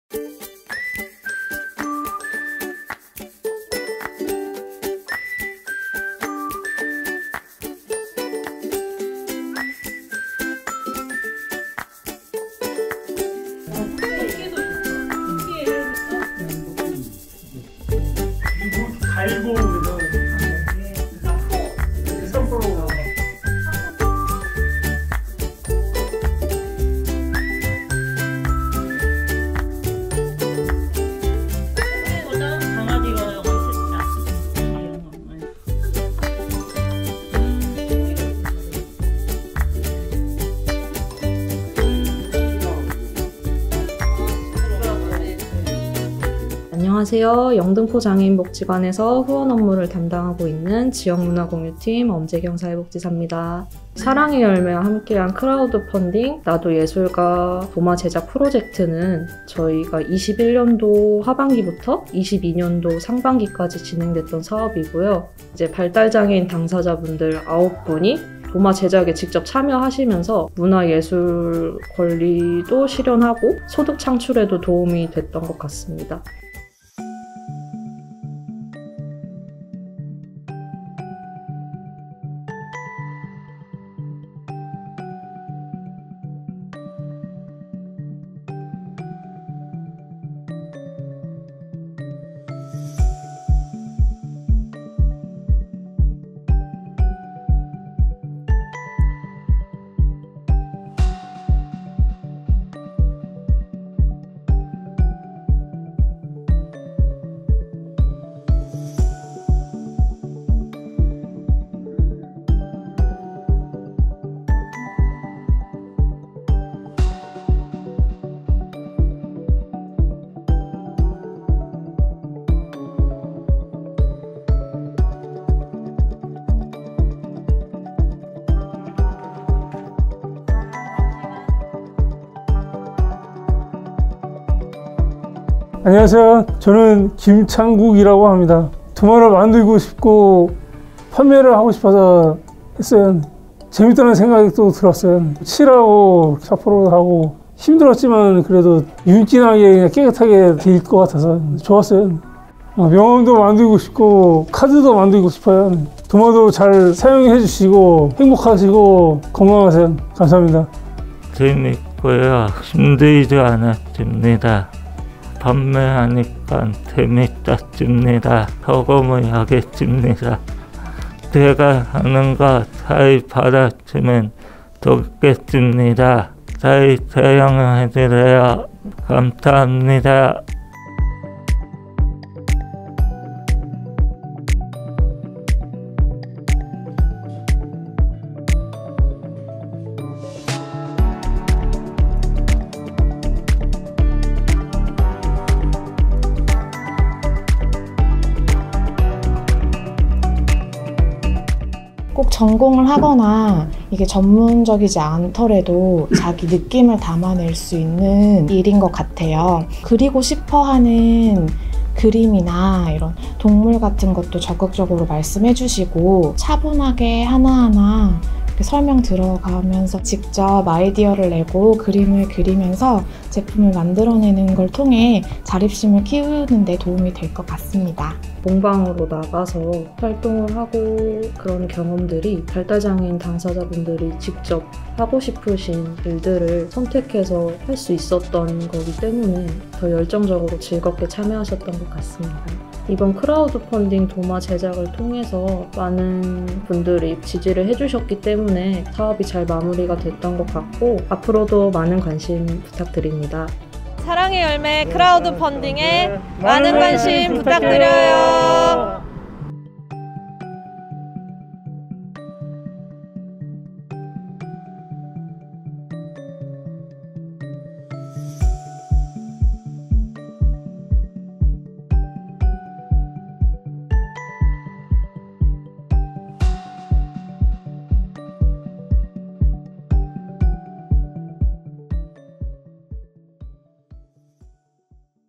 [사랑의열매와 함께하는 펀딩 모금 후기] 도마를 만드는 발달장애인 예술가들
사랑의열매 X 서울시립영등포장애인복지관

장애인의 자립, 취업을 위해 많은 변화와 움직임이 생기고 있지만, 여전히 장애인 평균 취업률은 34.9%, 그중 발달장애인 취업률은 23.2%에 그친다고 합니다.

사랑의열매는 서울시립영등포장애인복지관과 함께 발달장애인들의 문화 예술 권리를 보장하고, 소중한 꿈을 키울 수 있게 발달장애인 자립을 위한 '나도 예술가!' 지원사업을 진행했습니다.💕

83명의 기부자님들의 응원과 마음이 모여 총 3,497,140원의 기부금과 94%의 펀딩 달성률을 기록했고, 결과적으로 9명의 발달장애인 도마제작 공예를 배울 수 있었습니다.

도마를 제작하고 난 후 예비 예술가님들은 어떻게 지내고 계신지, 열매둥이가 직접 현장에 방문해 보았습니다!

함께 만나보시죠~!📢💕

사랑의열매 크라우드펀딩 이 궁금하시다면 지금 바로 아래 링크를 확인해주세요!

👉사랑의열매 크라우드펀딩 설명 보러가기